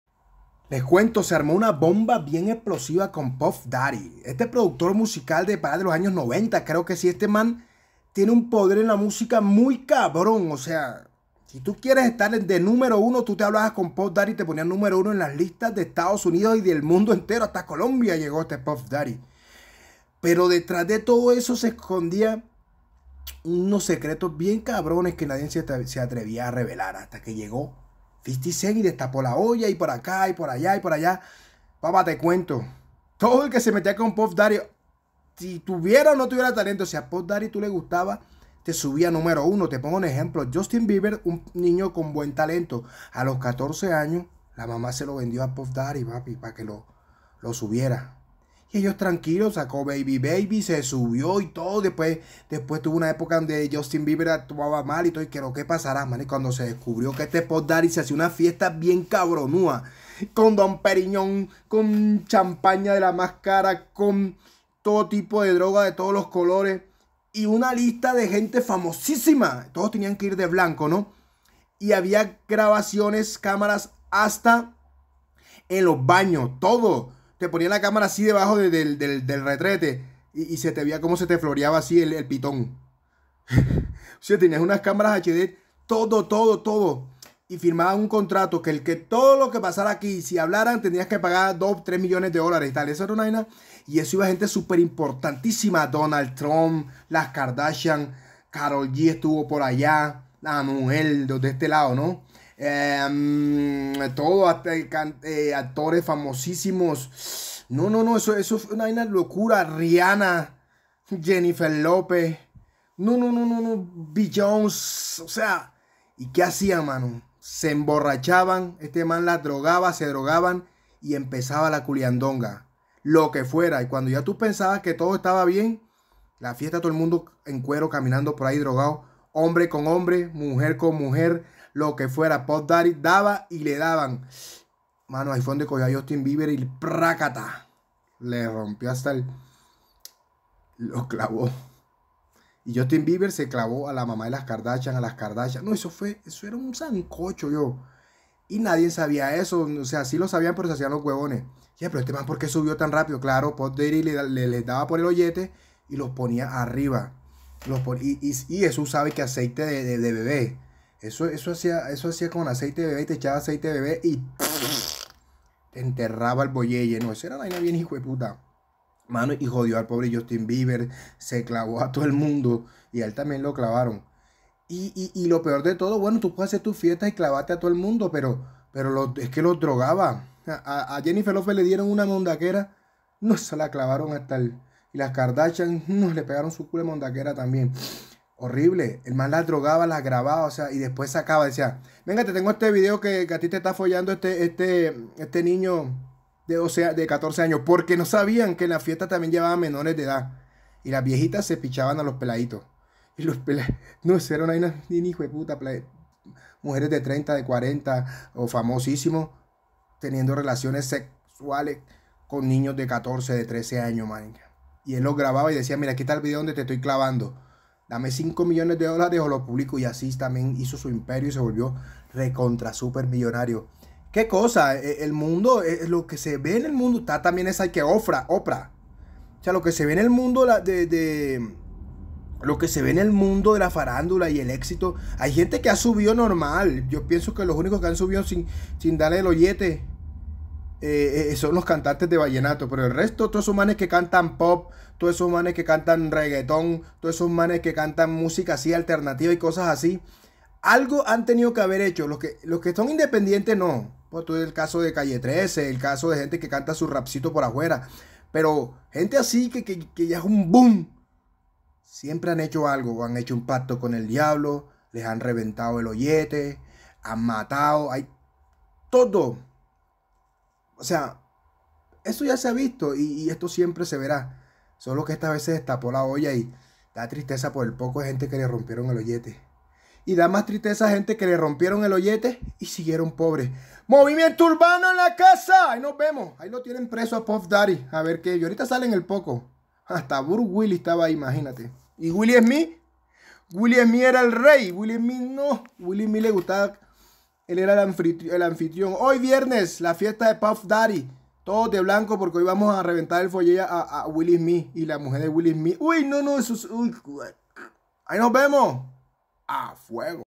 Puff daddy jennifer lopez. era complices depredadores sexuales de Hollywood
J lo y diddy usaban aceites de bebe 🍑